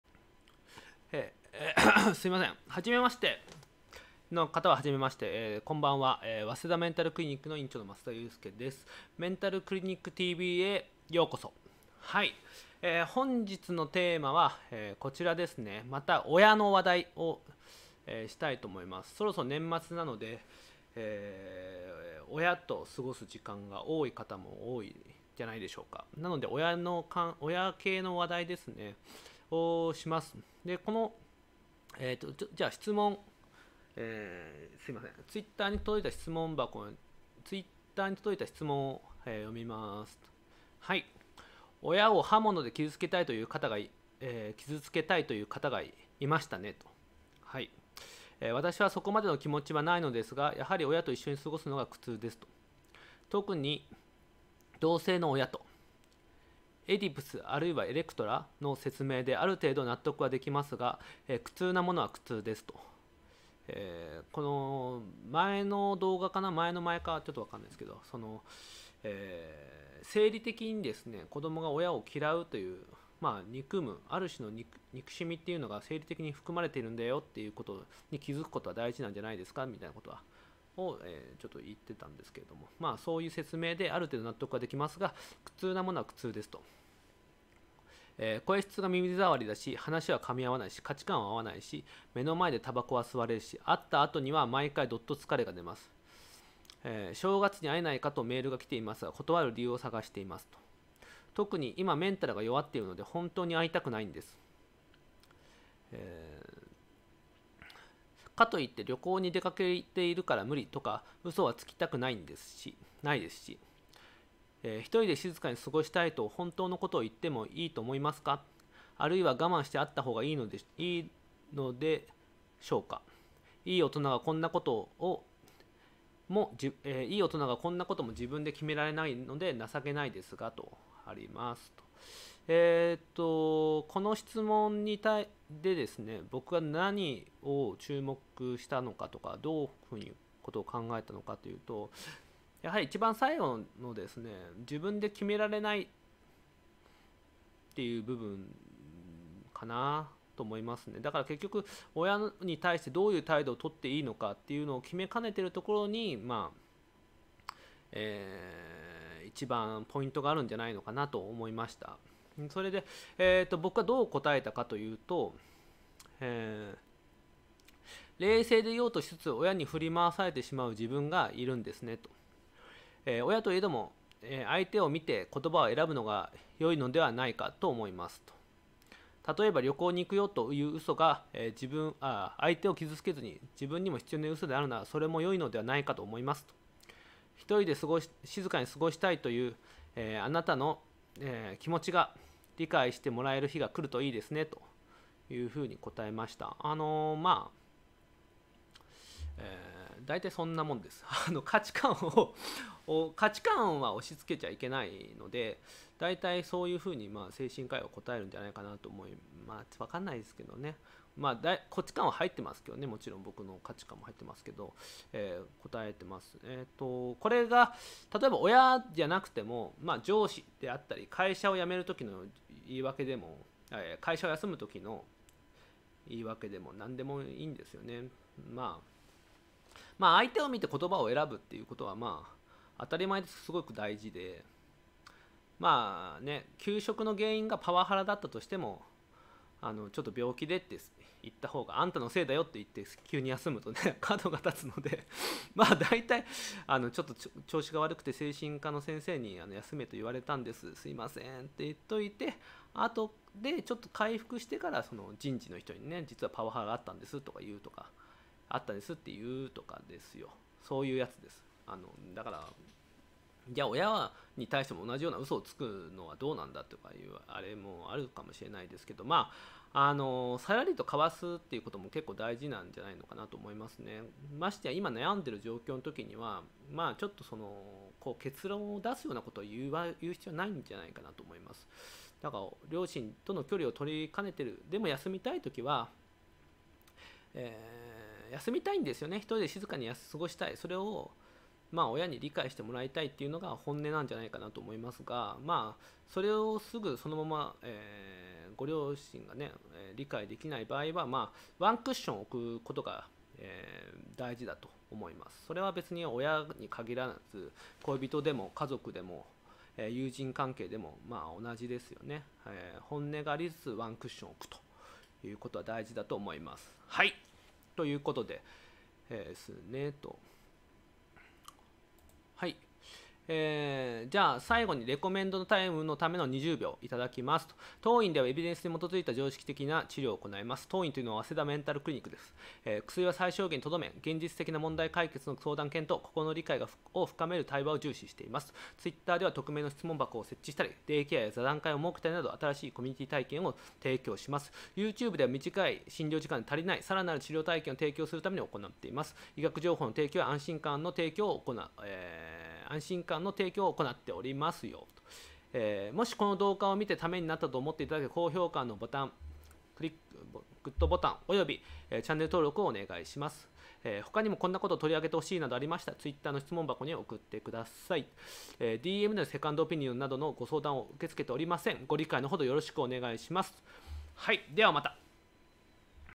年末年始に親に会いたくない時、どう答える？【精神科医・益田裕介/早稲田メンタルクリニック】
早稲田メンタルクリニック院長として働く、現役精神科医益田裕介が答えます！